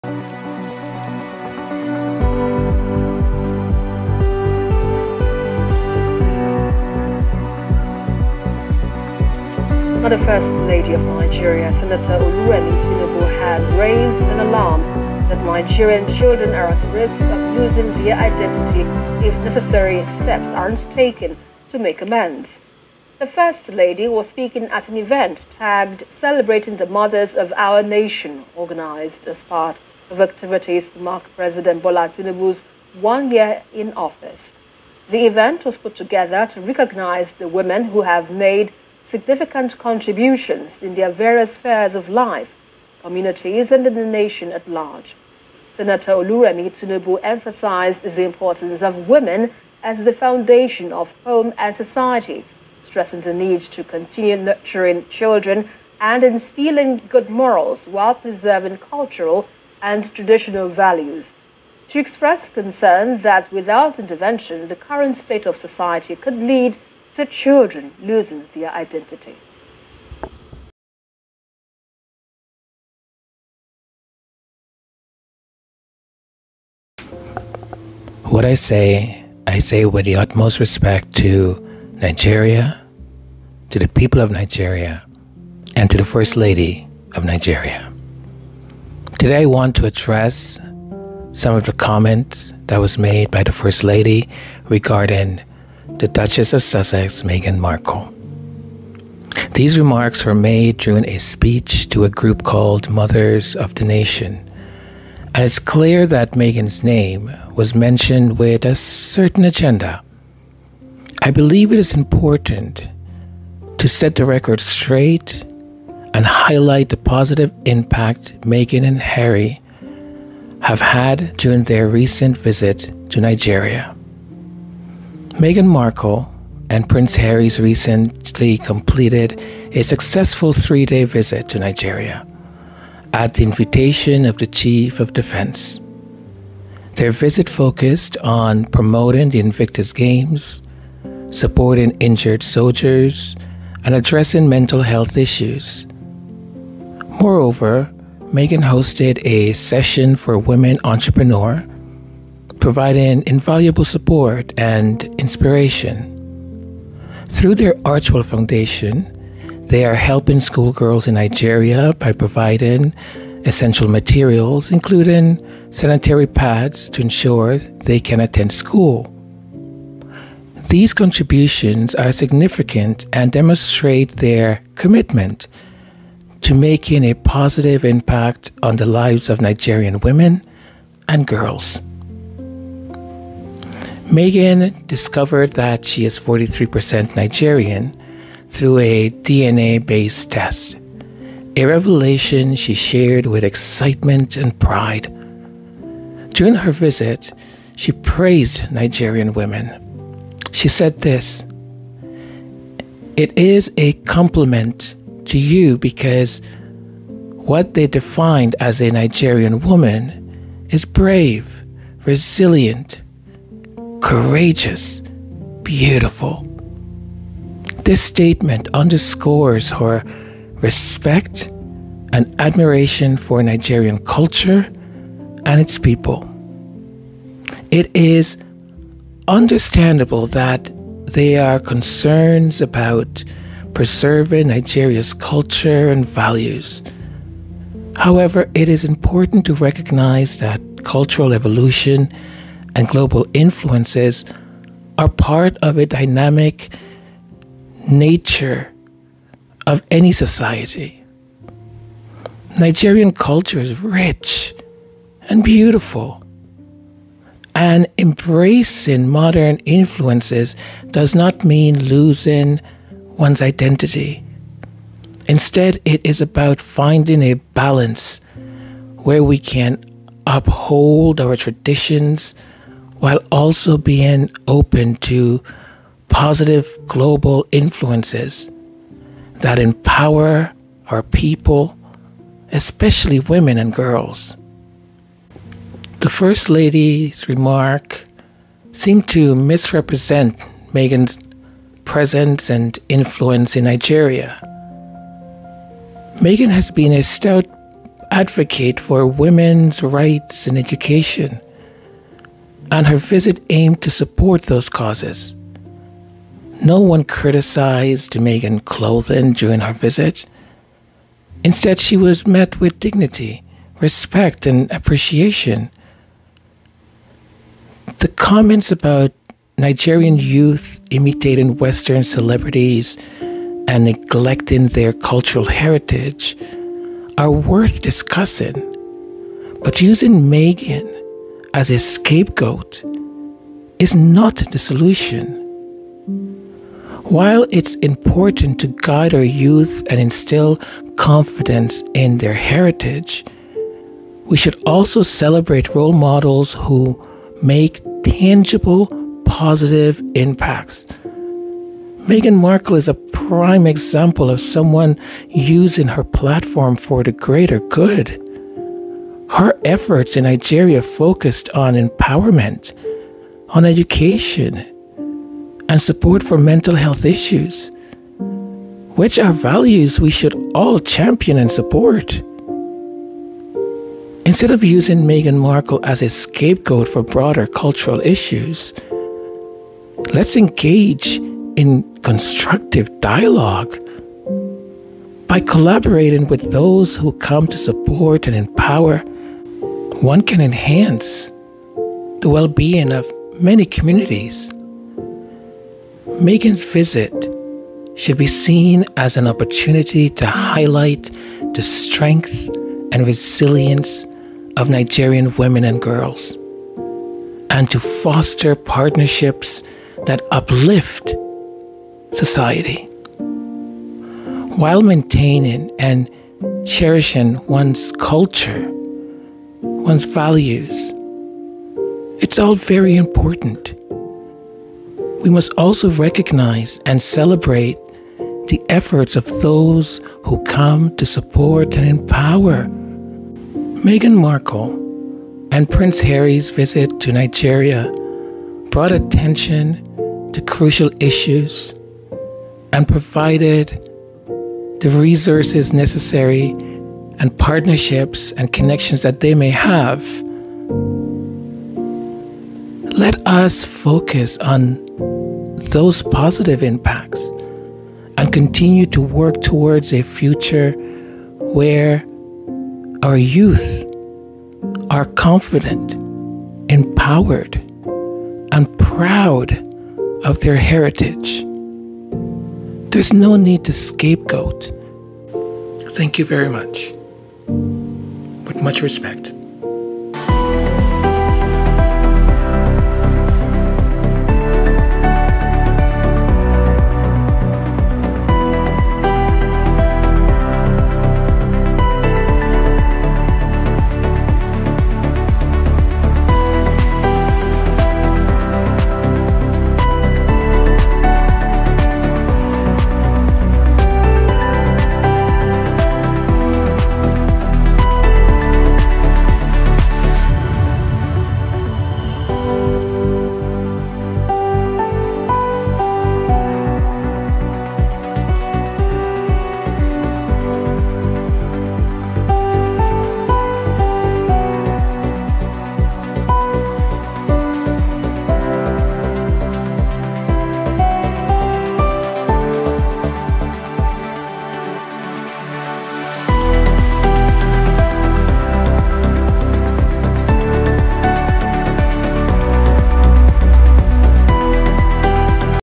Majesty Sussex Report - A RESPECTFUL RESPONSE TO THE FIRST LADY
THE FIRST LADY OF NIGERIA SURPRISINGLY DROPPED THE NAME OF MEGHAN MARKLE AS SHE WAS ABOUT TO COMPLETE HER SPEECH TO A WOMAN'S ORGANIZATION - "MOTHERS OF THE NATION". BY DOING SO SHE BROUGHT THE DUCHESS OF SUSSEX INTO NIGERIA'S INTERNAL CULTURAL ISSUES.